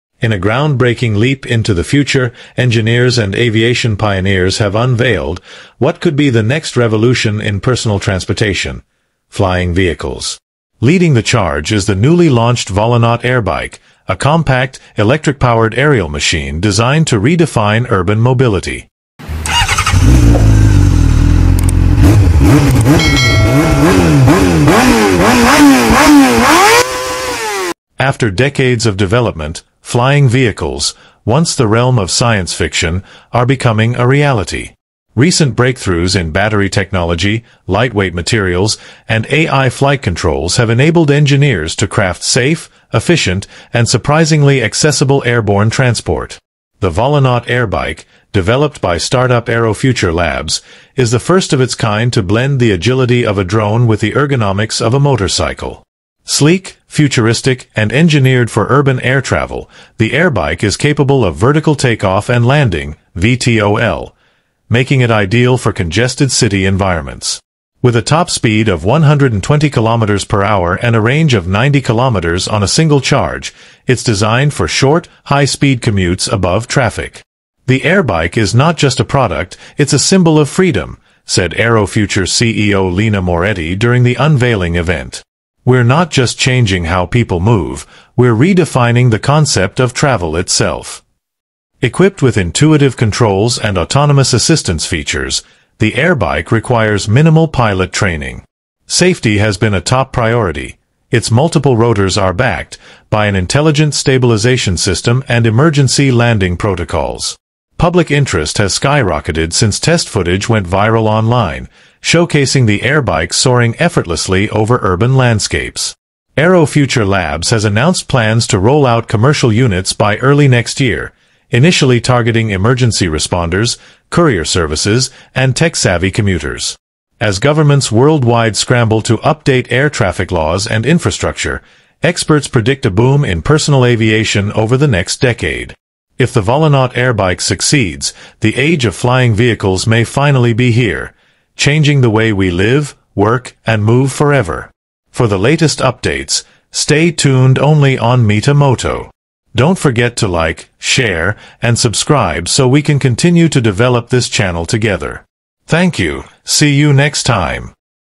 2025 NEW FLYING BIKE VOLONAUT LAUNCHED || INVENTION THAT CHANGED THE WORLD!!
2025 NEW FLYING BIKE VOLONAUT LAUNCHED || INVENTION THAT CHANGED THE WORLD!!

in a groundbreaking leap toward the future, engineers and aviation pioneers have unveiled what could be the next revolution in personal transportation: flying vehicles.
leading the way is the newly launched volonaut airbike, a compact, electric-powered aerial machine designed to redefine urban mobility.
after decades of development, flying vehicles—once confined to science fiction—are becoming a reality.
the latest breakthroughs in battery technology, lightweight materials, and ai flight control have enabled engineers to create air transport that is safe, efficient, and highly accessible.